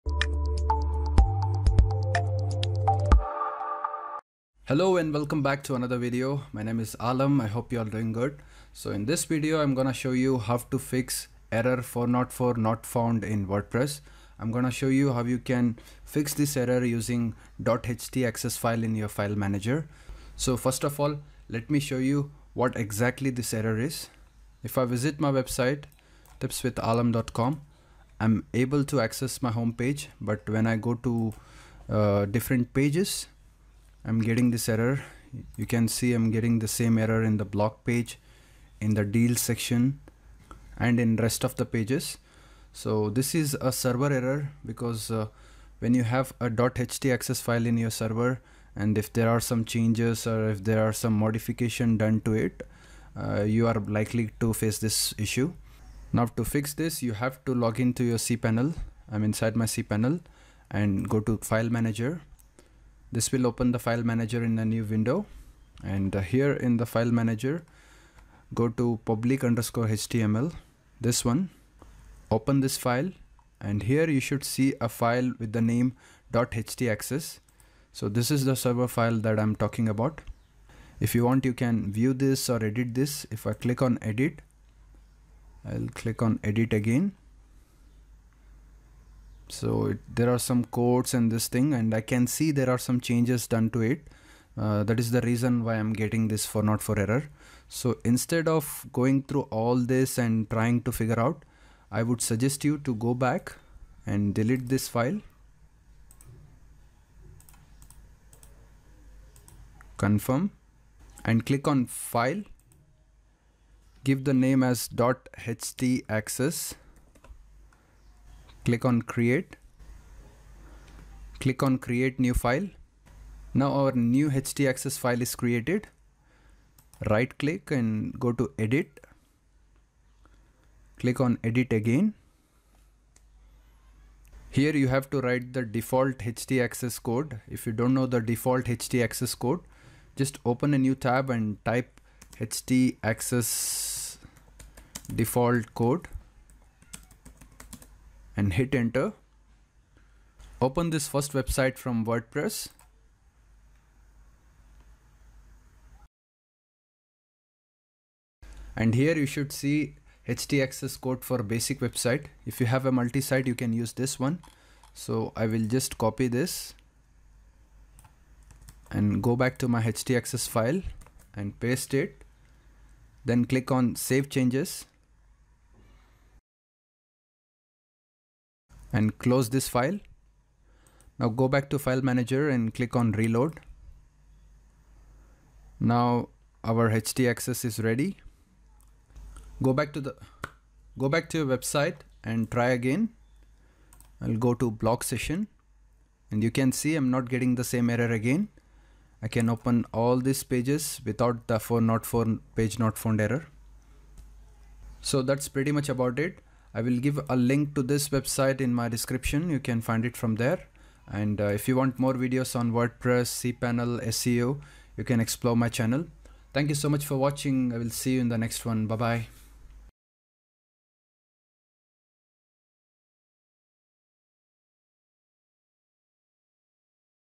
How To Fix 404 Page Not Found Error In Wordpress
About this video:
In this WordPress tutorial, I show you How To Fix 404 Page Not Found Error In Wordpress websites. The HTTP 404, 404 Not Found, 404, 404 Error, Page Not Found or File Not Found error message is a Hypertext Transfer Protocol (HTTP) standard response code, in computer network communications, to indicate that the browser was able to communicate with a given server, but the server could not find what was requested. I've shown you how to edit .htacess file & fix 404 page not found error in wordpress.

TIMESTAMPS
00:00 Introduction
00:56 How To Fix 404 Page Not Found Error In Wordpress
01:17 How to edit .htacess file in file manager (cpanel)